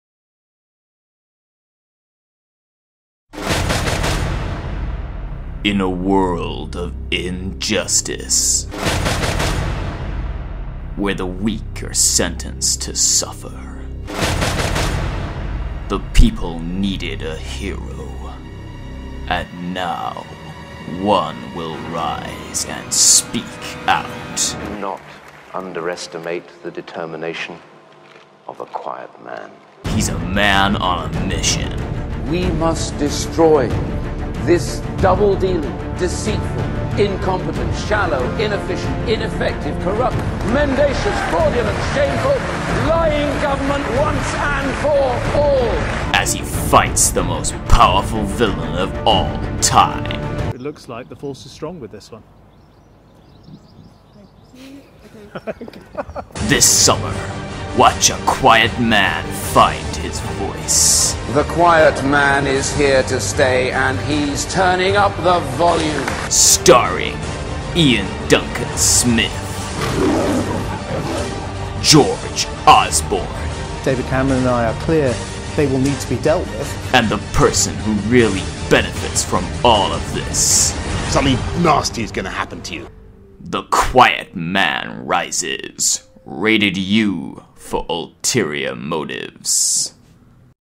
The Quiet Man Rises Trailer
Iain Duncan Smith is to star in a new summer blockbuster